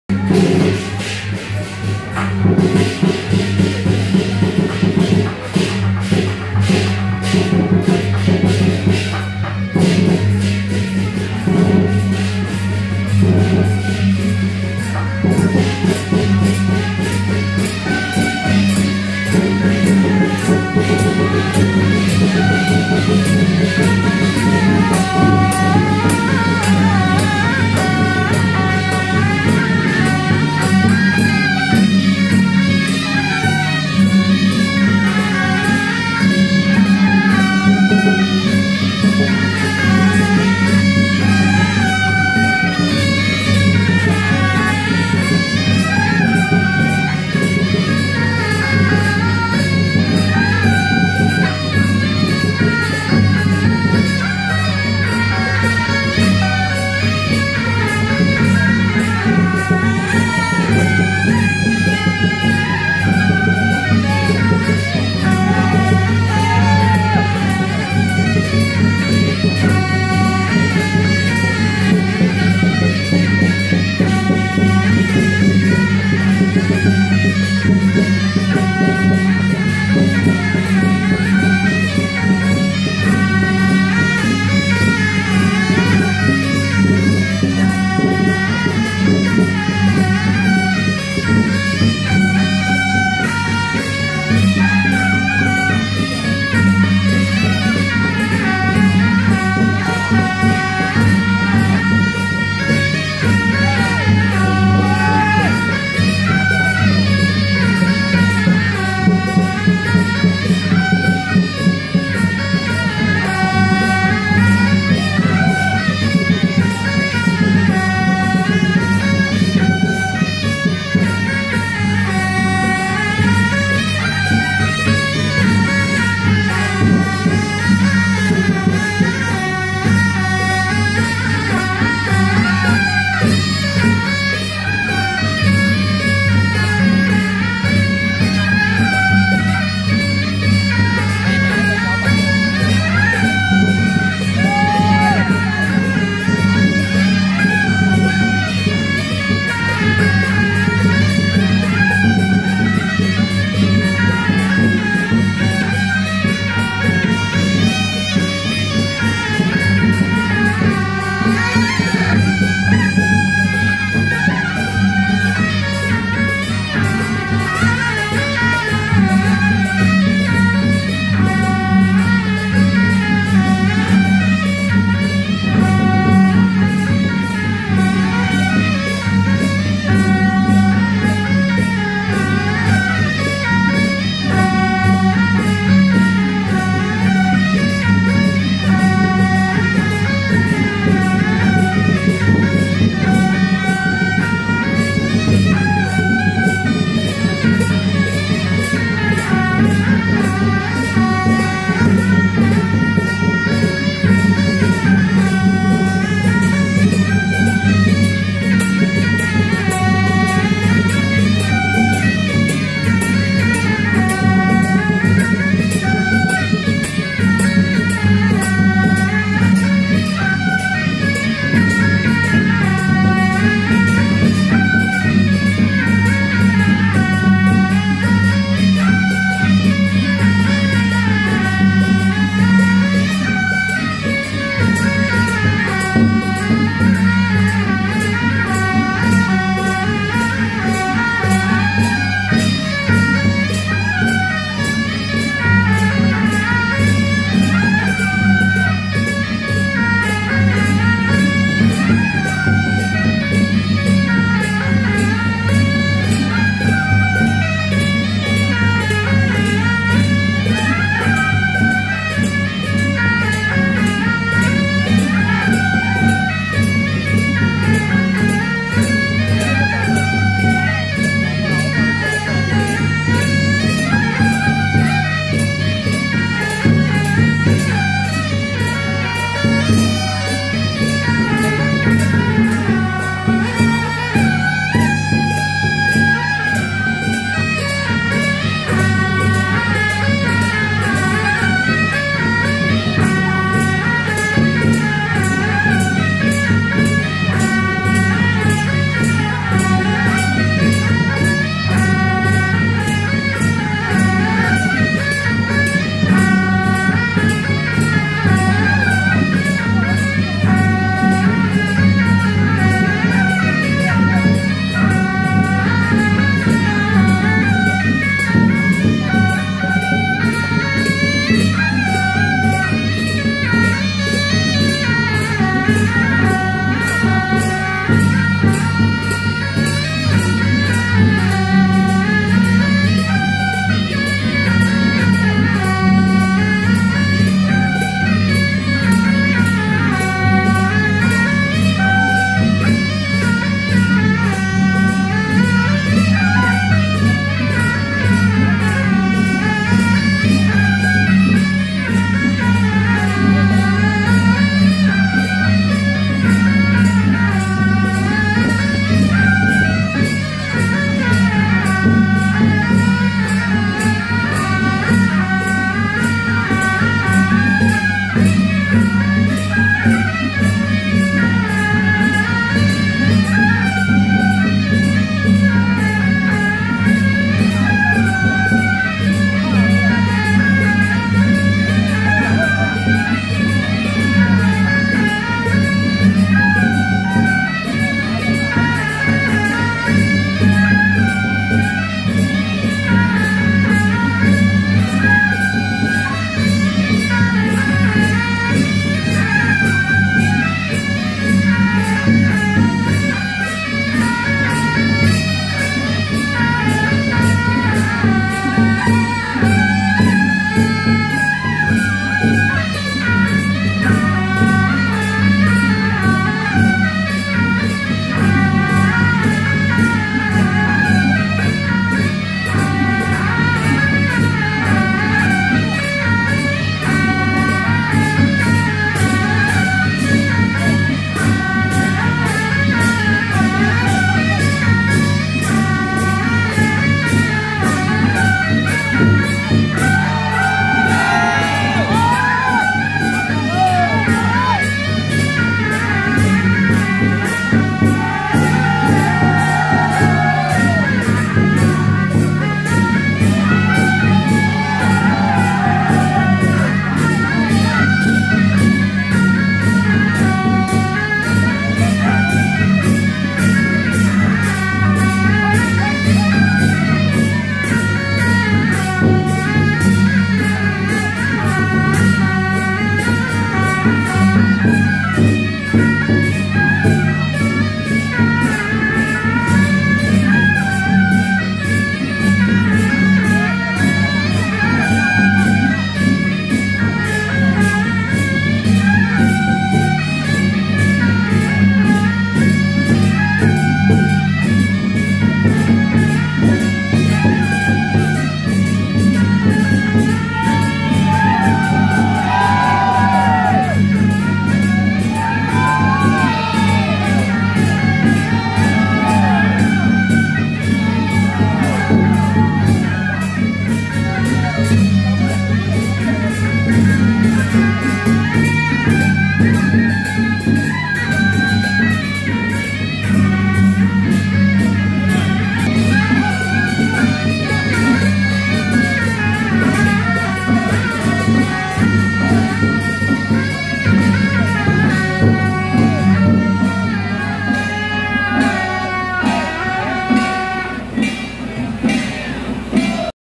【大溪鳳山寺廣澤宮-神將團＆主帥轎~保安廣澤尊王(三太保) 聖駕】~111大溪鳳山寺廣澤宮桃聯廣澤會舘保安廣澤尊王南巡會香回駕遶境
台北南安社-北管＆大溪鳳山寺廣澤宮-神將團＆主帥轎~保安廣澤尊王(三太保) 聖駕